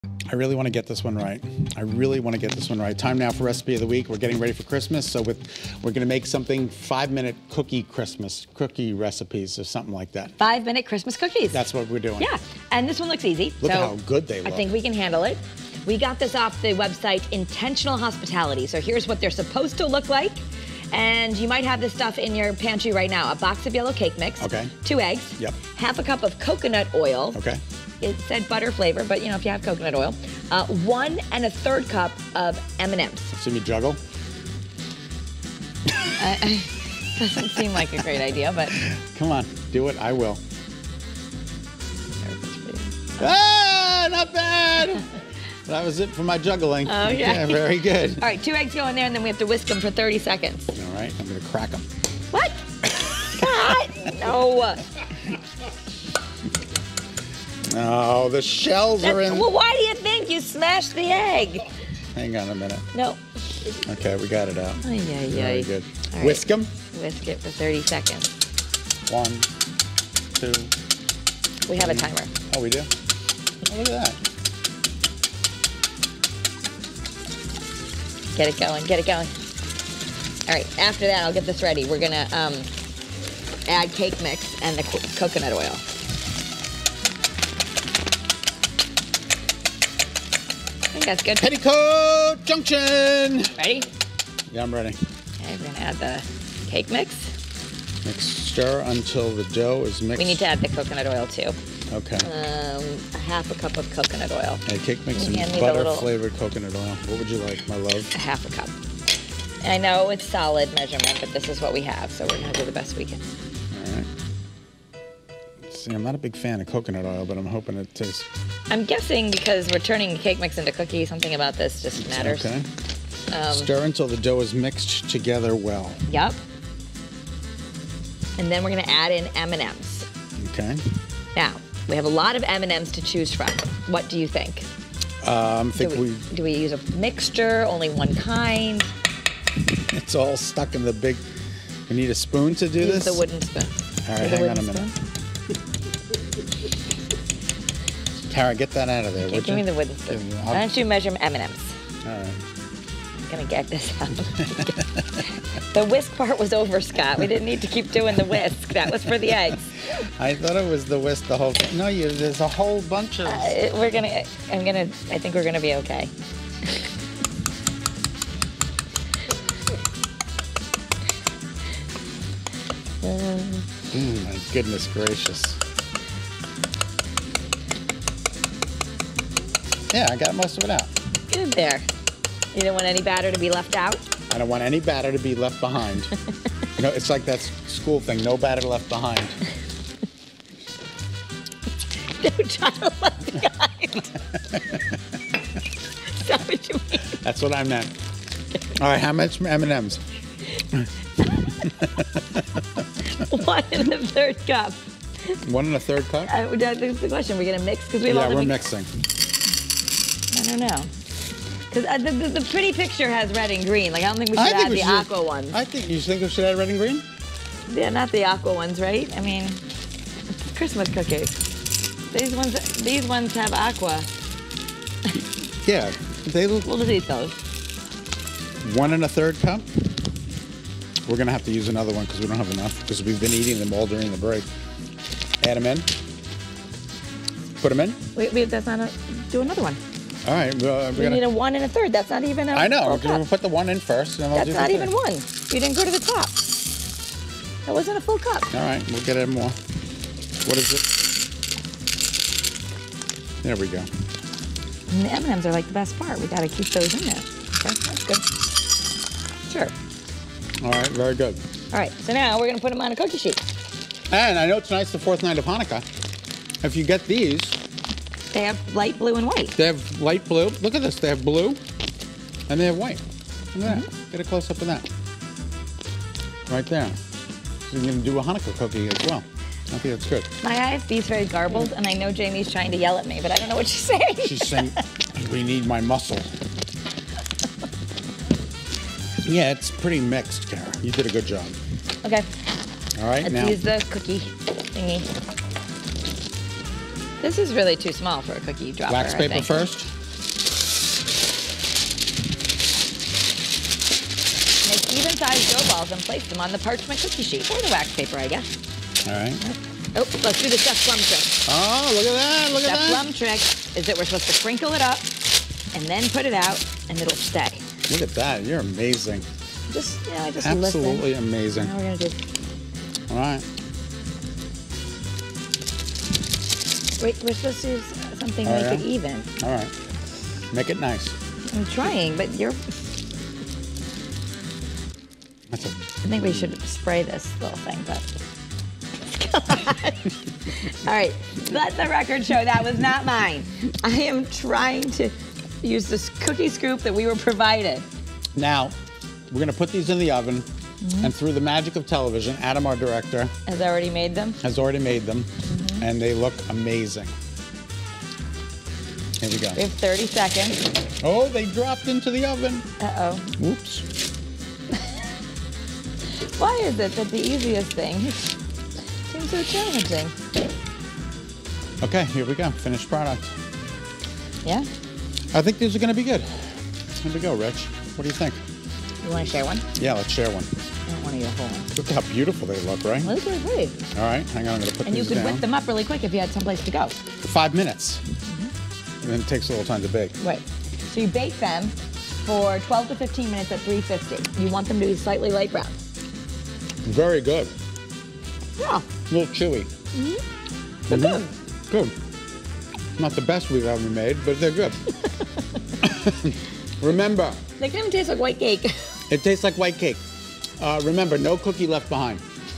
Recipe of the Week: 5 Minute Christmas Cookies
Scot and Kara are making 5 minute Christmas cookies!  The recipe is from the website Intentional Hospitality.  The ingredients are: 1 box of yellow cake mix, 2 eggs, 1/2 cup of coconut oil, 1 1/3 cup M&Ms.  You'll find the directions below.